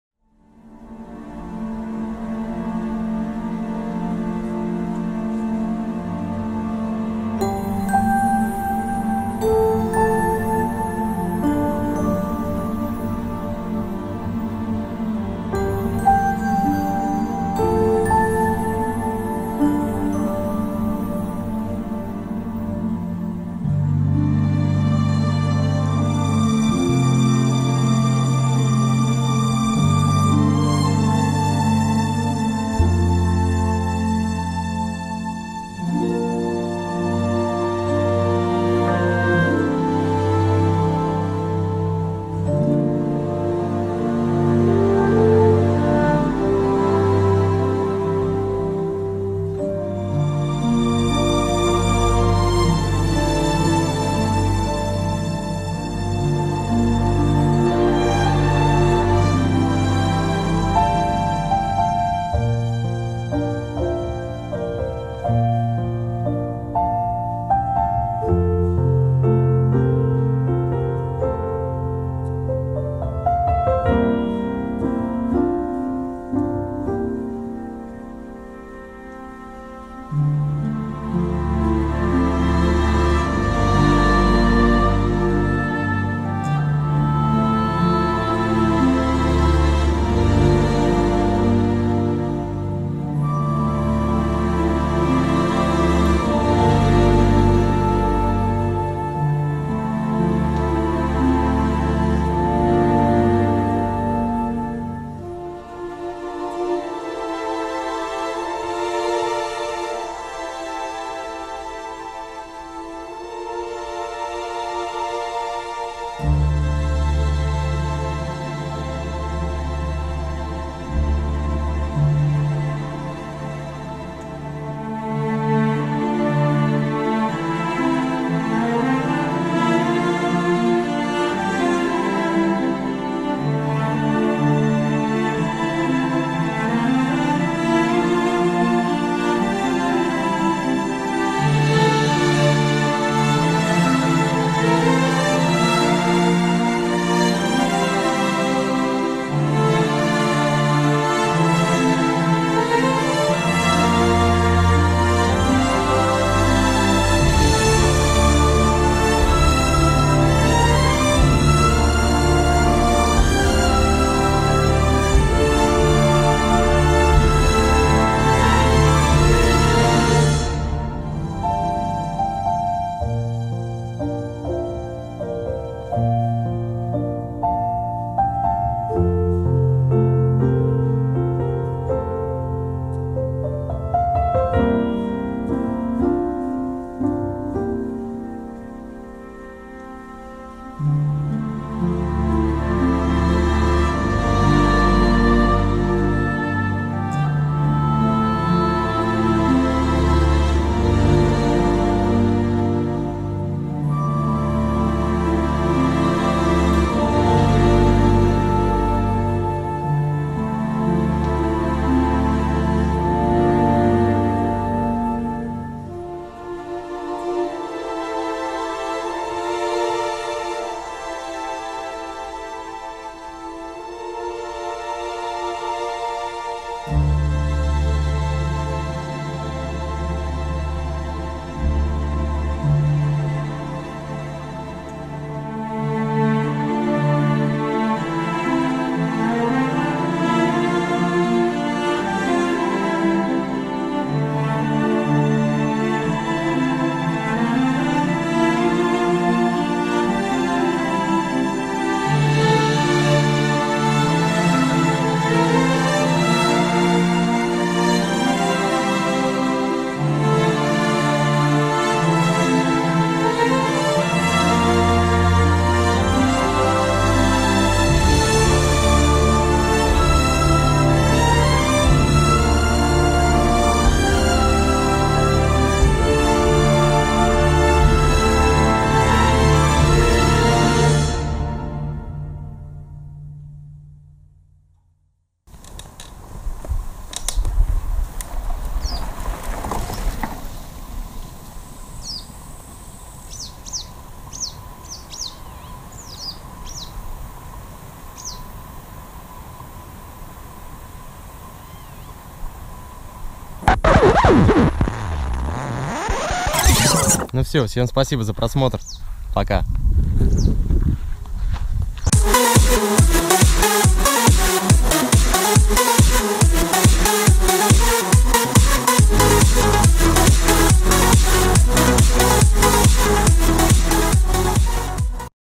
RUSSIA FULL HD, DRONE, UNTOUCHED TERRITORIES
I just want to share the video that I made last year during my trip home.